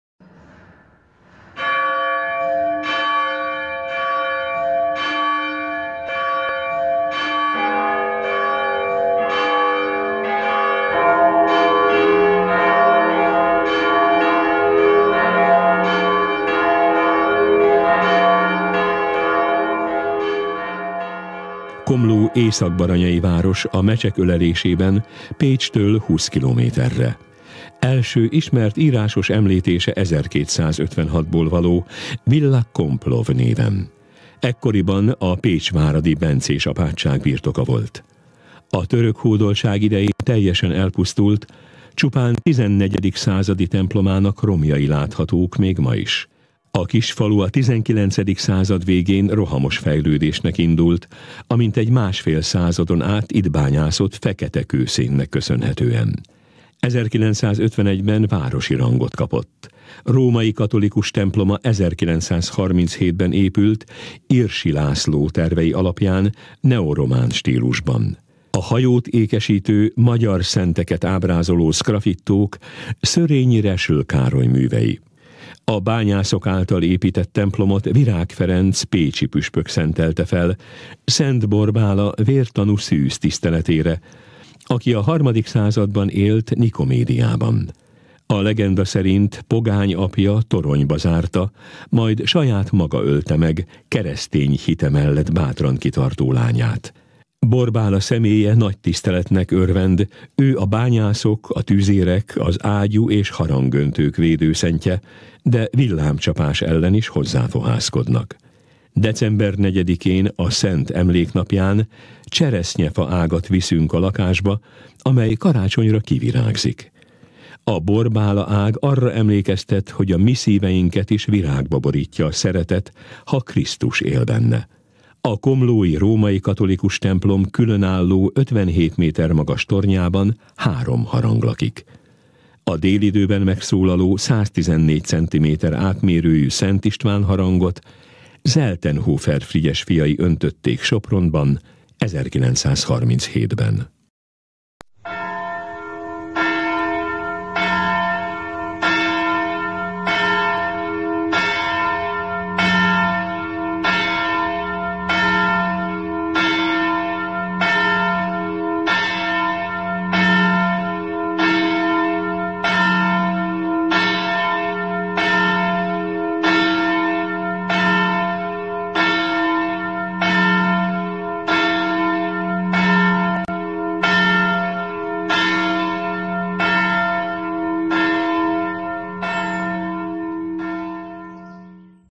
A komlói nagyharang - Déli harangszó (2020. 12. 04.)
Tisztelet a bányászok és a bányászat védőszentje előtt

Varga Péter komlói nyugdíjas főaknász kezdeményezésére a komlói római katolikus templom nagyharangjának hangja szólt Szent Borbála napján, délben a Kossuth Rádióban.

“Komló észak-baranyai város a Mecsek ölelésében, Pécstől 20 kilométerre. Első ismert írásos említése 1256-ból való Villa Complov néven. Ekkoriban a pécsváradi bencés apátság birtoka volt. A török hódoltság idején teljesen elpusztult, csupán XIV. századi templomának romjai láthatók még ma is.

A kis falu a XIX. század végén rohamos fejlődésnek indult a mintegy másfél századon át itt bányászott feketekőszénnek köszönhetően. 1951-ben városi rangot kapott. Római katolikus temploma 1937-ben épült Irsy László tervei alapján, neoromán stílusban. A hajót ékesítő, magyar szenteket ábrázoló sgrafittók Szörényi-Reischl Károly művei.

A bányászok által épített templomot Virág Ferenc pécsi püspök szentelte fel Szent Borbála vértanú szűz tiszteletére, aki a III. században élt Nikomédiában. A legenda szerint pogány apja toronyba zárta, majd saját maga ölte meg keresztény hite mellett bátran kitartó lányát. Személye nagy tiszteletnek örvend, ő a bányászok, a tüzérek, az ágyú- és harangöntők védőszentje, de villámcsapás ellen is hozzá fohászkodnak.

December 4-én, a szent emléknapján, cseresznyefaágat viszünk a lakásba, amely karácsonyra kivirágzik. A Borbála-ág arra emlékeztet, hogy a mi szíveinket is virágba borítja a szeretet, ha Krisztus él benne.

A komlói római katolikus templom különálló, 57 méter magas tornyában három harang lakik. A délidőben megszólaló, 114 centiméter átmérőjű Szent István harangot Seltenhofer Frigyes fiai öntötték Sopronban, 1937-ben” (Kossuth Rádió, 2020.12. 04.)